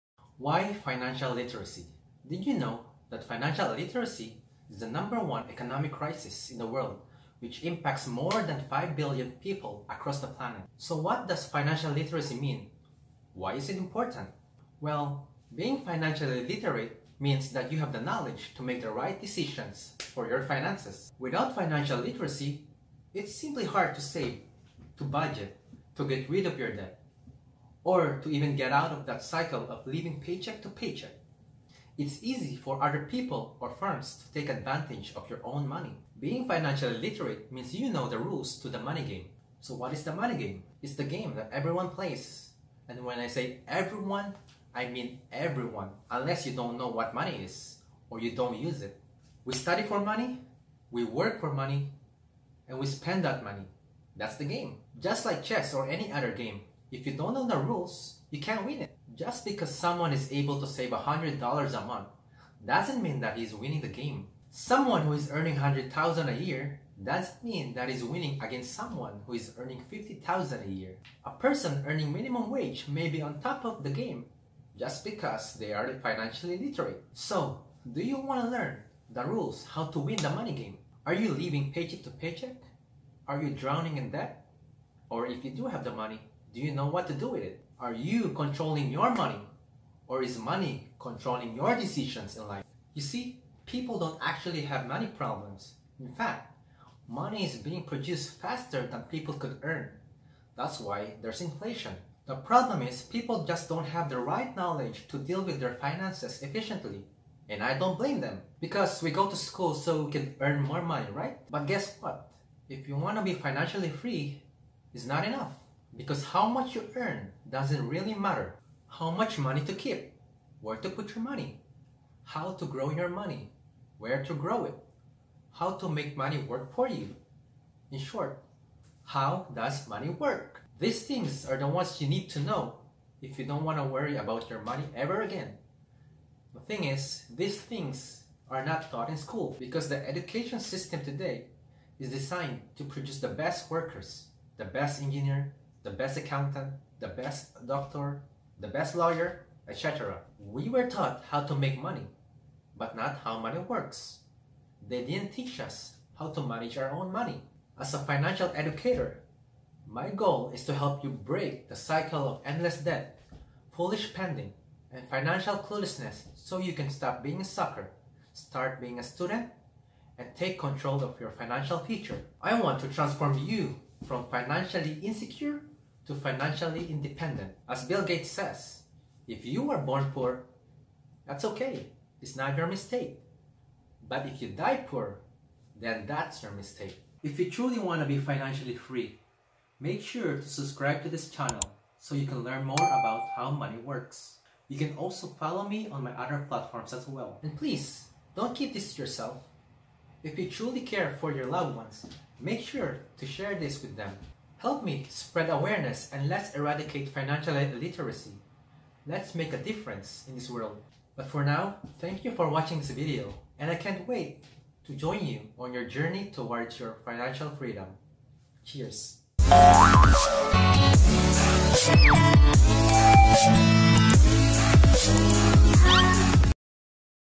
What is the #1 Reason for Your Money Problems? | How Much Money You Make Doesn't Matter
What is the #1 reason for your money problems. Why are money problems so common?

Are you financially struggling? Are you drowning in debt? Are you living paycheck to paycheck?

How much money you make or how much responsibilities you have doesn't matter when it comes to your financial standing.

Financial freedom does not depend on your income or your financial responsibilities.

If you want to win the money game, you need to know the rules to the game. You need to know how money works which is never taught in school.

watch the video below to learn more.